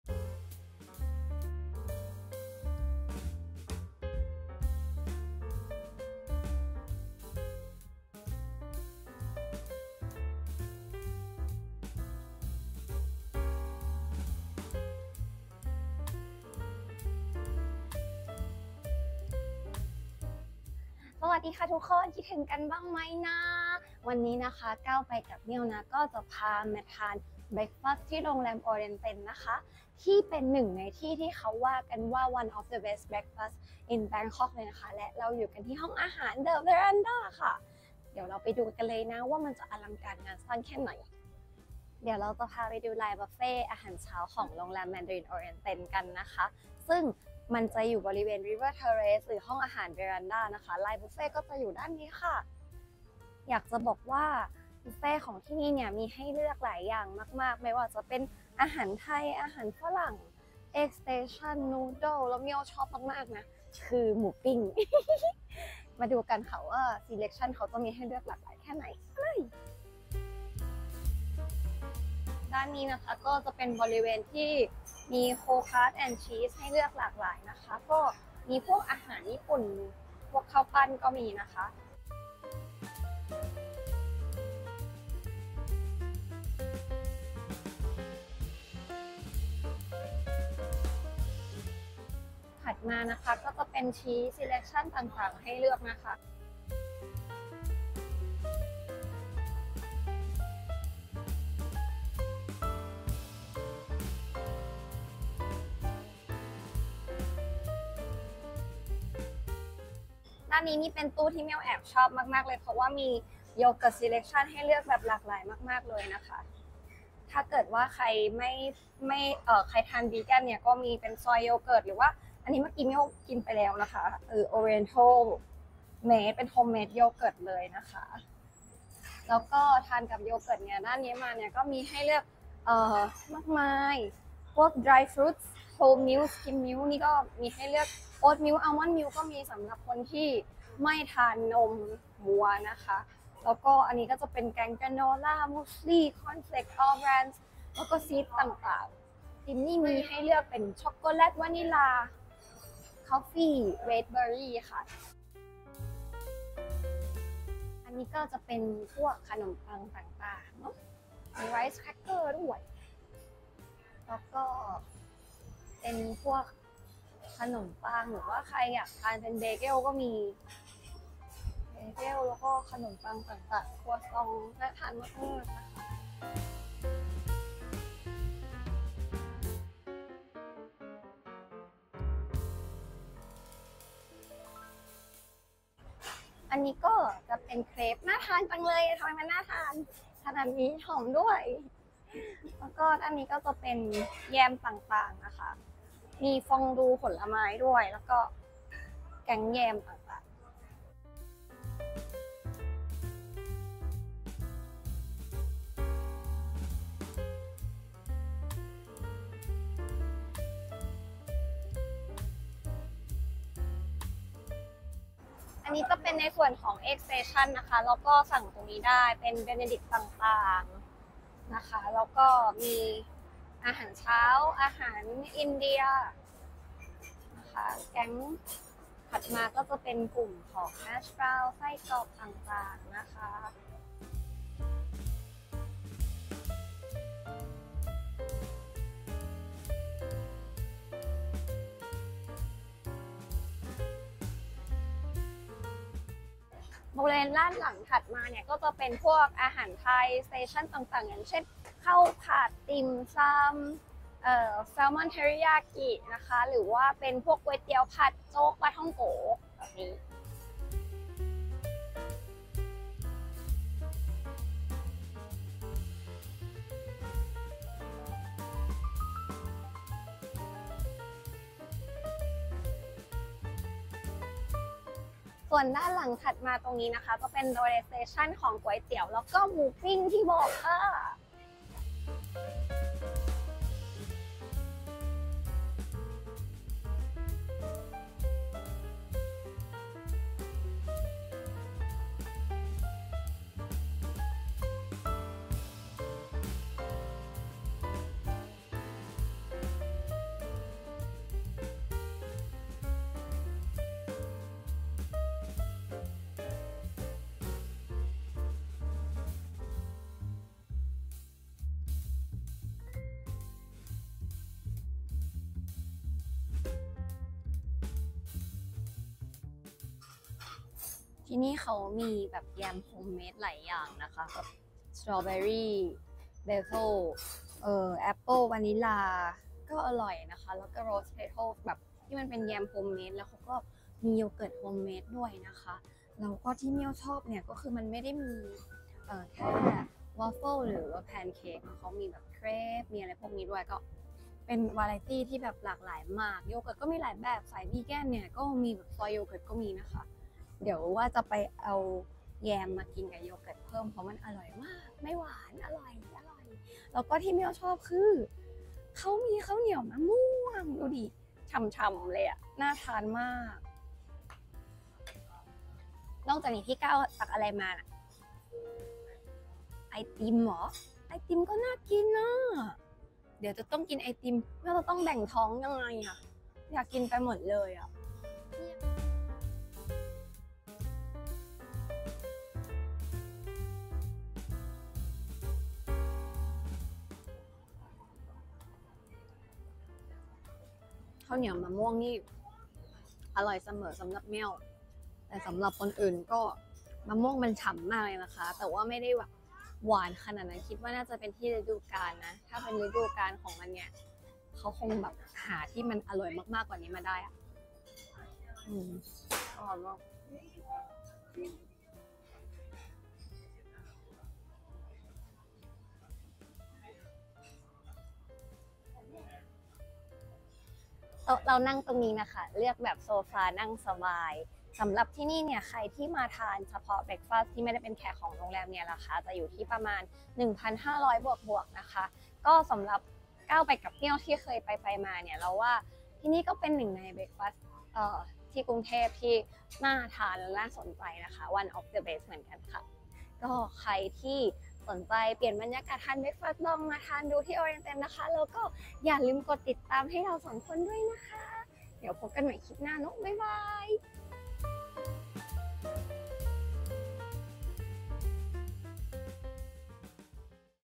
บุฟเฟต์อาหารเช้า ที่ Verandah โรงแรม Mandarin Oriental Bangkok สุดอลังการอาหารแน่นมาก
บุฟเฟต์อาหารเช้าที่โรงแรม Mandarin Oriental Bangkok ดีงามมากอาหารคุณภาพแบบแน่นๆ ทานได้แบบไม่อั้น คุ้มค่าเกินราคากับวิวแม่น้ำเจ้าพระยาสุดโรแมนติก

อย่าลืมติดตามช่อง "9ไปกับเมี่ยว" ด้วยนะครับ